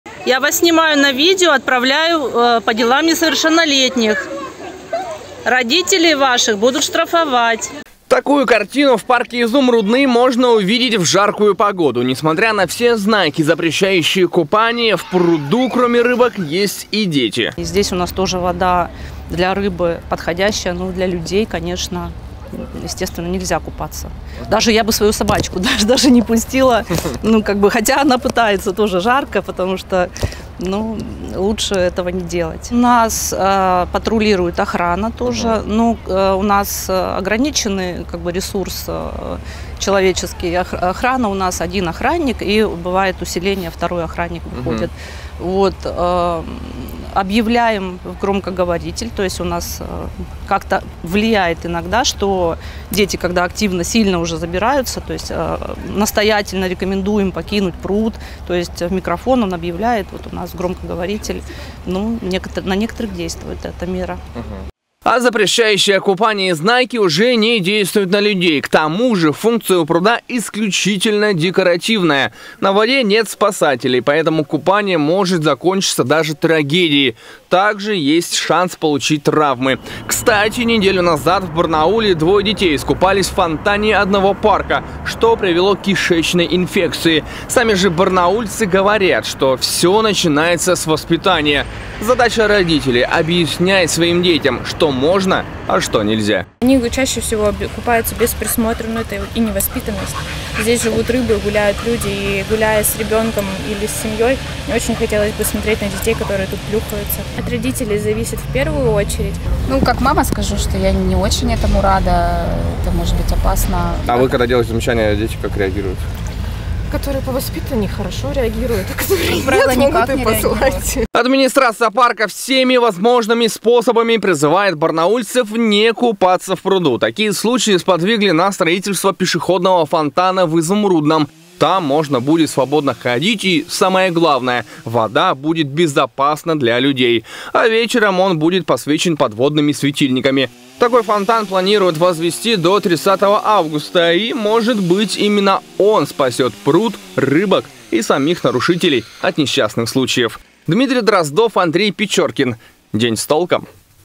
Жители Барнаула никак не перестанут купаться в пруду в парке "Изумрудный"
Жители города Барнаула продолжают жаловаться на купающихся детей в пруду парка "Изумрудный". Это продолжается на протяжении всего лета. Несмотря на все замечания, рейды, разъяснительные беседы – в жаркую погоду в пруду многолюдно. А это – небезопасно.